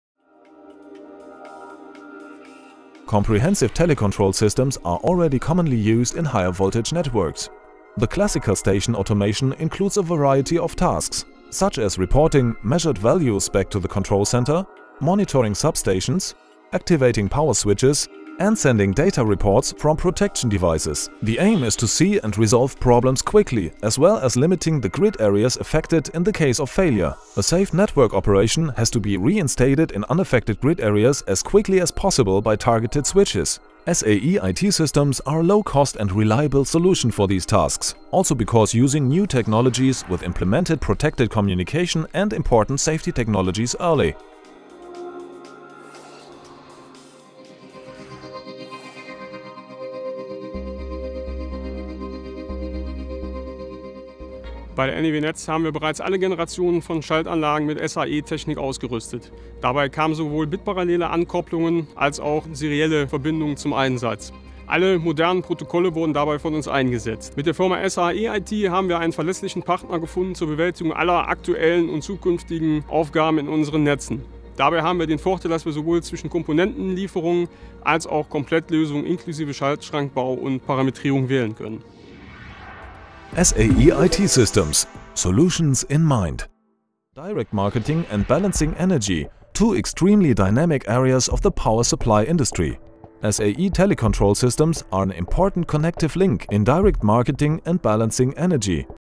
Substation automation systems | Substation control technology
► Substation automation systems ✓ Substation control technology ✓ ► SAE IT-systems - Solutions in Mind ► Your leading manufacturer for high-performance telecontrol and substation automation technology.

Extensive telecontrol equipment has long been standard in higher voltage ranges. Here, substation automation systems encompasses many different tasks. These include transmitting measured values to the control centre, monitoring transformers, activating circuit breakers and transmitting data from protective equipment.

Besides detecting problems fast, our goal is to limit the number of power grid regions affected in the event of a fault. Therefore, a safe network status must be restored in the unaffected power sections as quickly as possible.

SAE solutions also provide comprehensive IT security features that conform to the BDEW White Paper on Security in Power Systems and the ISMS Guide.